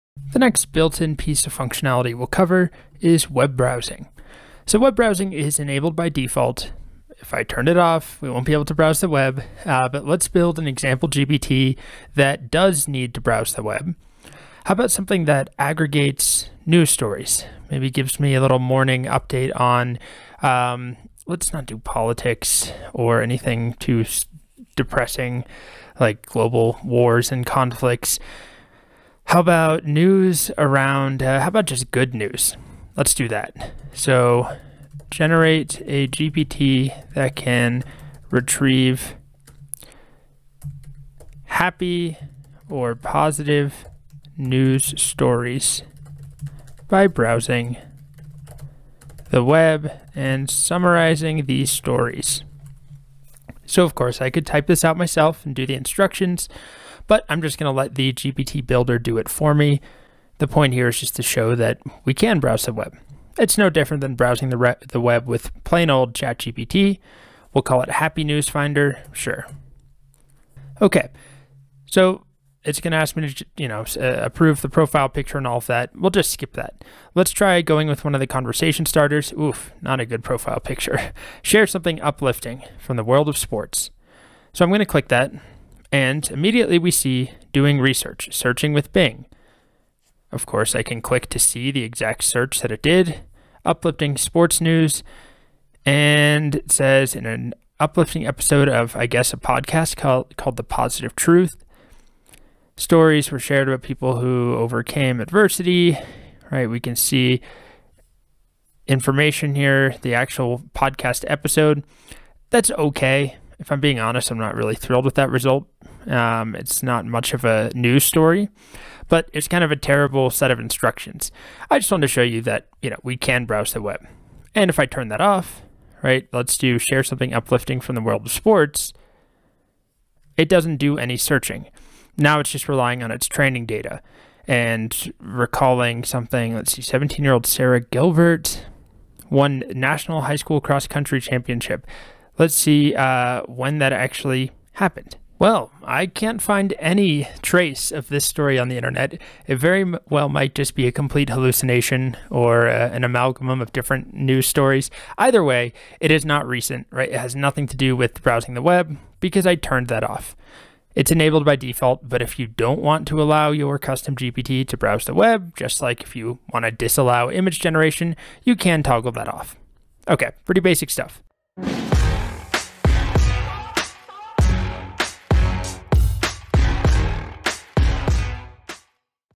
Web Browsing With Custom GPTs
Get ready to embark on an exciting journey into the realm of web browsing with custom GPTs! In this video, we delve deep into the techniques, secrets, and potential of utilizing custom GPTs for an enhanced browsing experience. Join us as we unlock the mysteries and reveal the magic behind web browsing with custom GPTs. Don't miss out on this transformative opportunity!